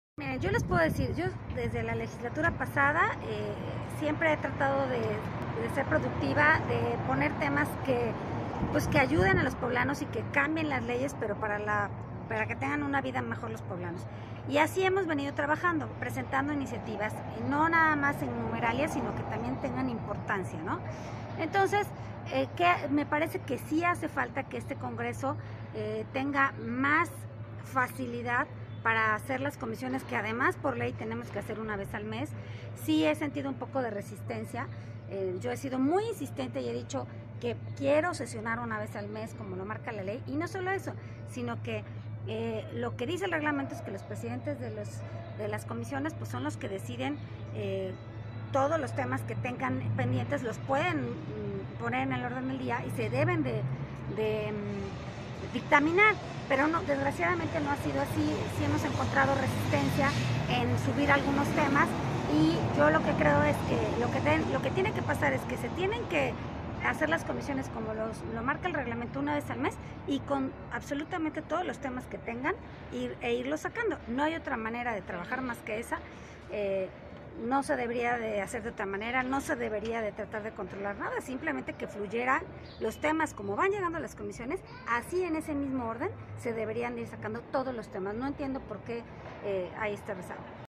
Mónica Rodríguez Della Vecchia reconoce que el Congreso de Puebla está rezagado
La diputada local del PAN, Mónica Rodríguez Della Vecchia, reconoce que el
Congreso de Puebla está rezagado en la discusión de iniciativas, ya que solo han votado 13 en comisiones. Señala que existe resistencia de los presidentes de comisiones para enlistar reformas.